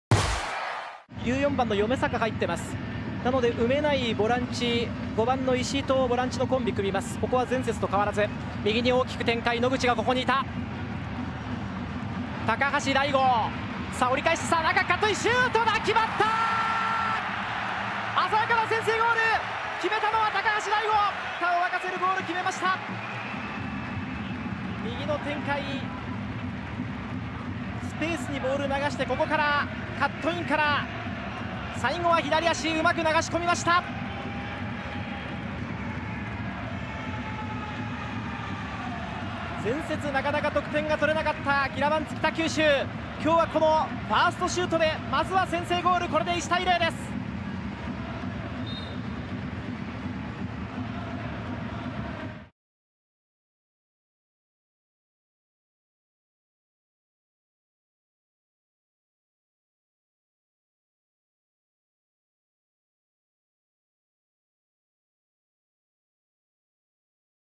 2019年10月度のＪ３月間ベストゴールは髙橋 大悟（ギラヴァンツ北九州）に決定！
２０１９明治安田生命Ｊ３リーグ 10月度の月間ベストゴールの動画です。
第26節 10/13（日） ギラヴァンツ北九州 vs いわてグルージャ盛岡の2分に生まれたギラヴァンツ北九州の髙橋 大悟選手によるゴールに決定しました。

●受賞者コメント
このような賞をいただけて嬉しいですし、初めてのことなので忘れられないものになると思いますし、また取れるように頑張りたいです。ゴールについては、試合の立ち上がりだったので思い切ってシュートを打った結果、入ってくれてよかったと思います。残り試合も昇格・優勝のために全力で頑張りたいと思います！